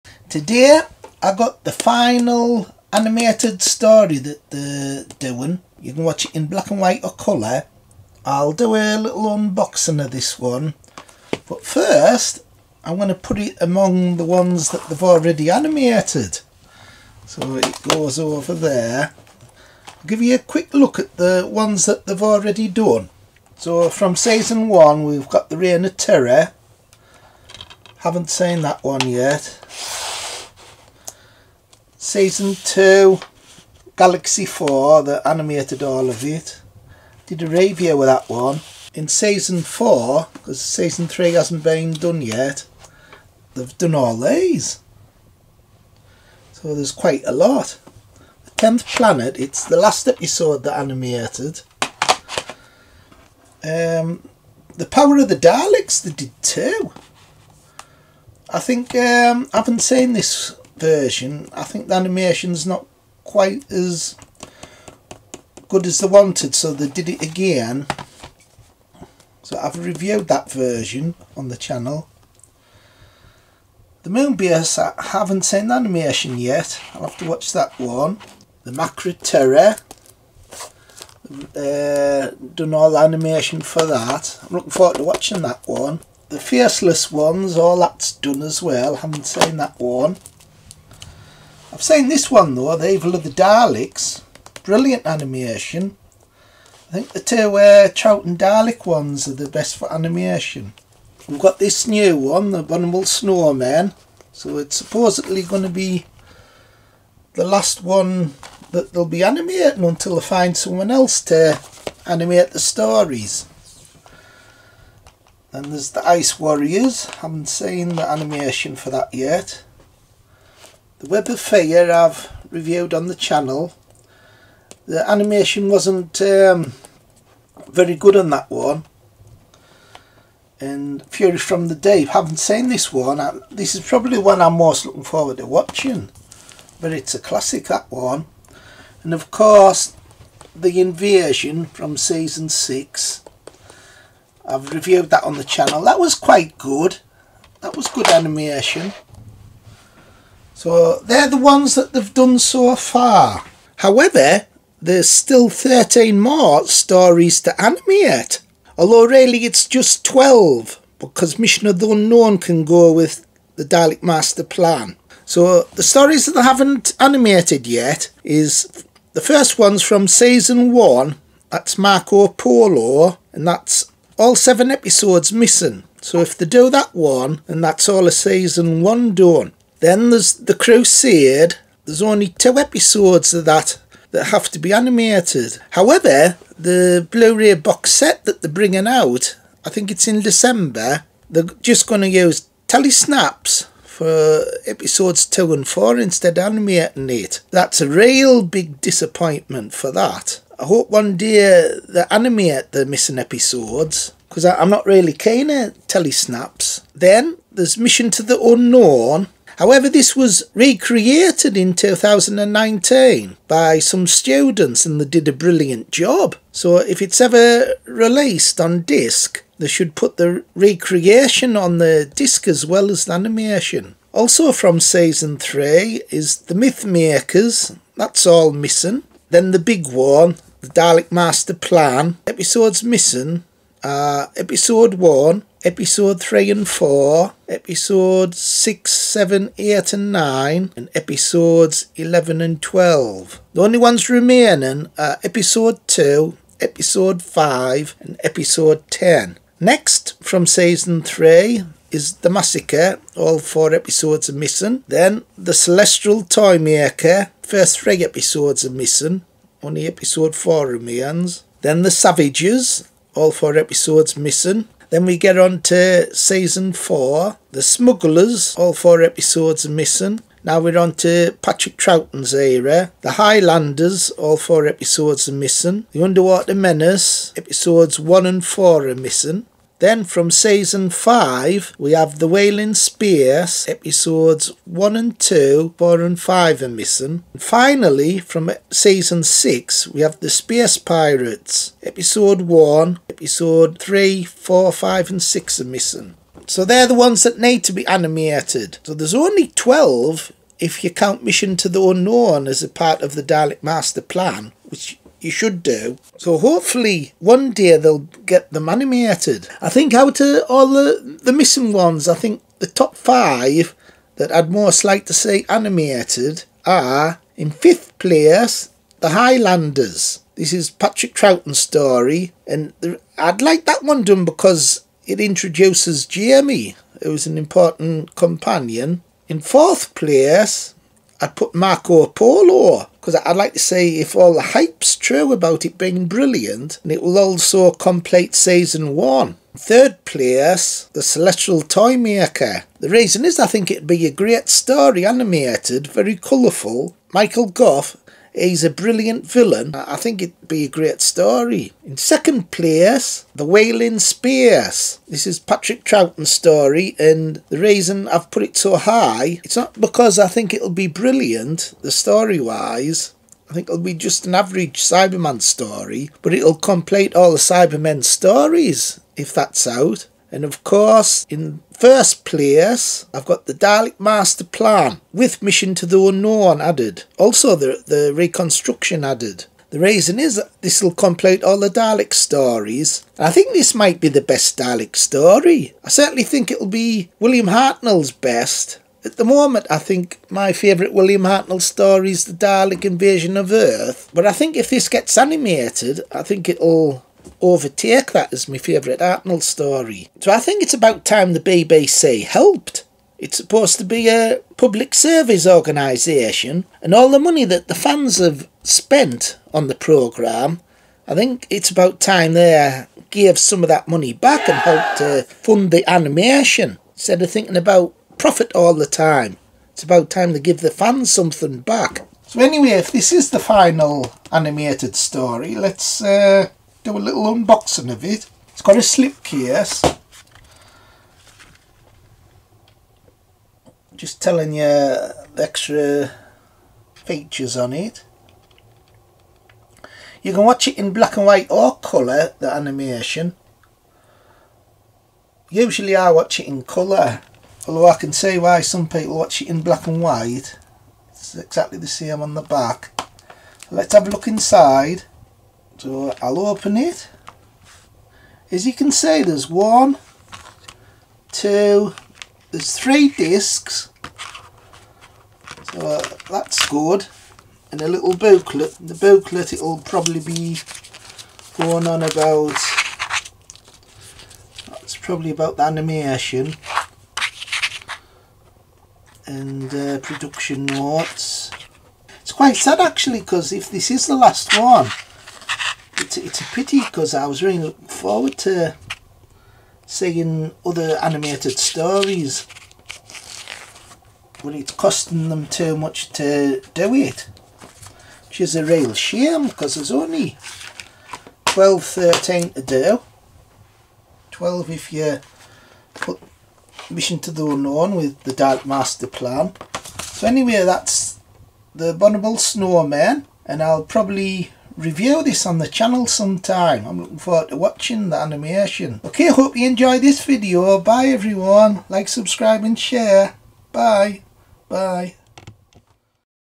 DOCTOR WHO: MISSING EPISODES TO ANIMATE
Marco Polo
The Crusade
Mission to the Unknown
The Myth Makers
The Daleks' Master Plan
The Massace
The Celestial Toymaker
The Savages
The Smugglers
The Highlanders
The Underwater Menace
The Wheel in Space
The Space Pirates